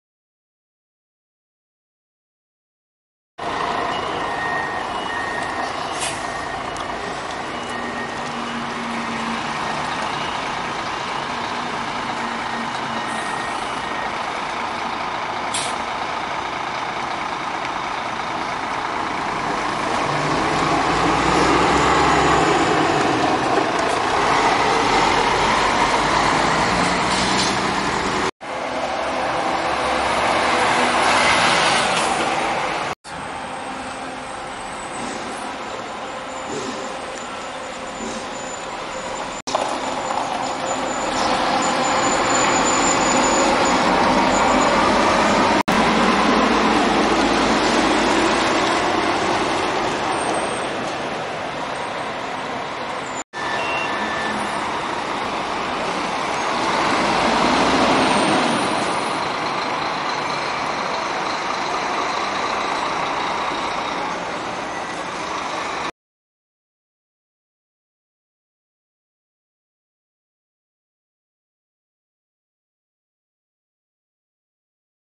EXPRESSO do SUL (VIAÇÃO COMETA) - Marcopolo Paradiso G7 1200 - MB O-500RSD
Ônibus EXPRESSO do SUL (VIAÇÃO COMETA) - Marcopolo Paradiso G7 1200 - Mercedes Benz O-500RSD